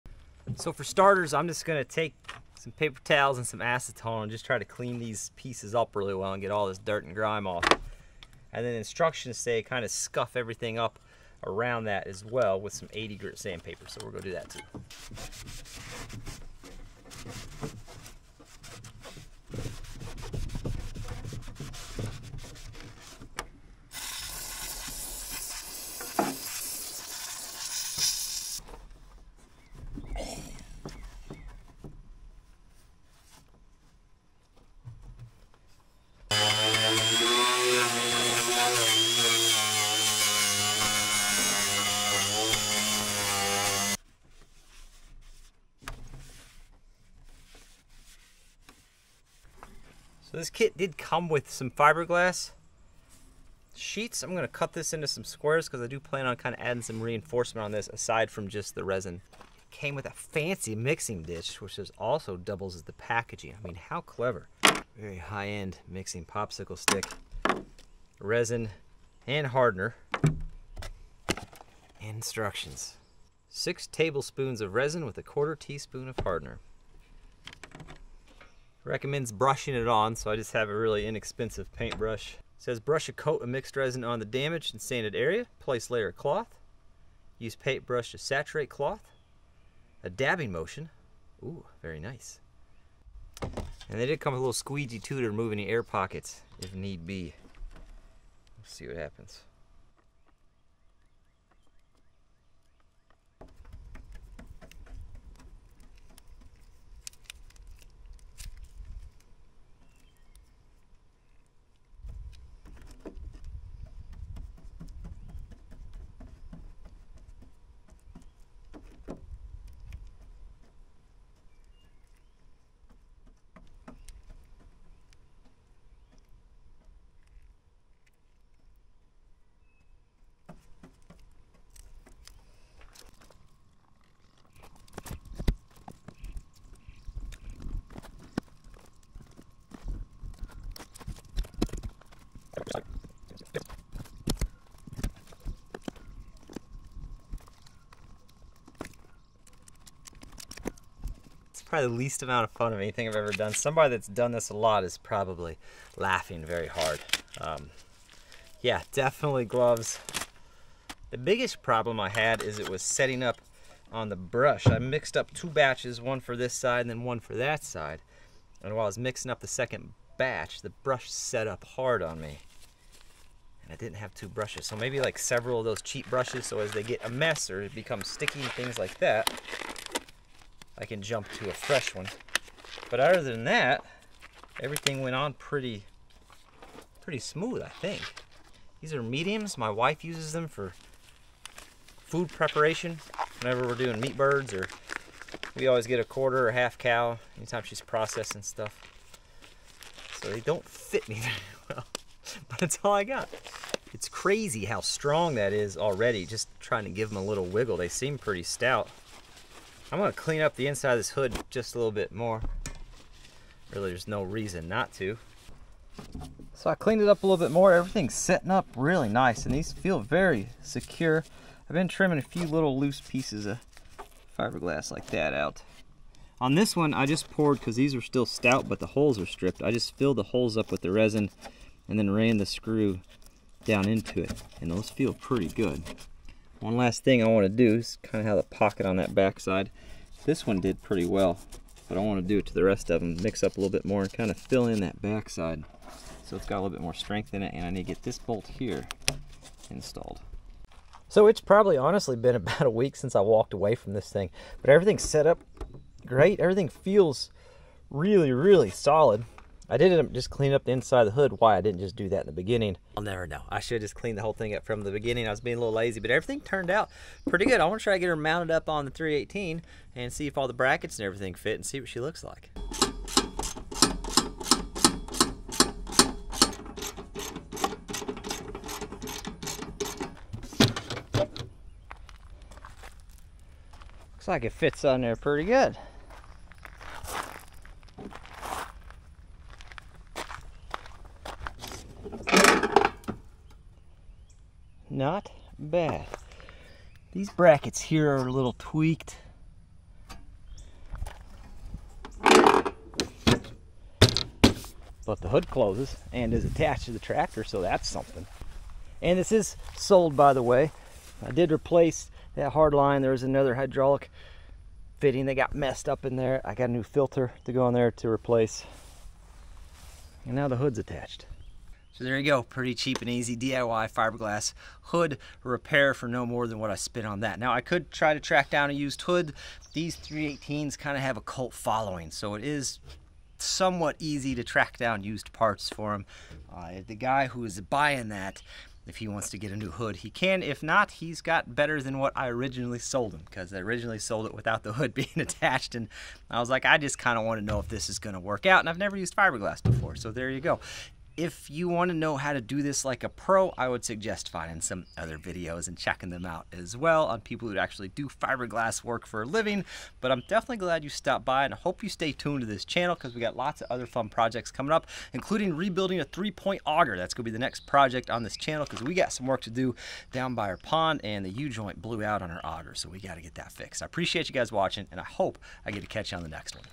DIY Guide: Fixing a Cracked Lawn Tractor Hood with Fiberglass Repair!
Is repairing a john deere fiberglass hood better than replacing it?  Find Out!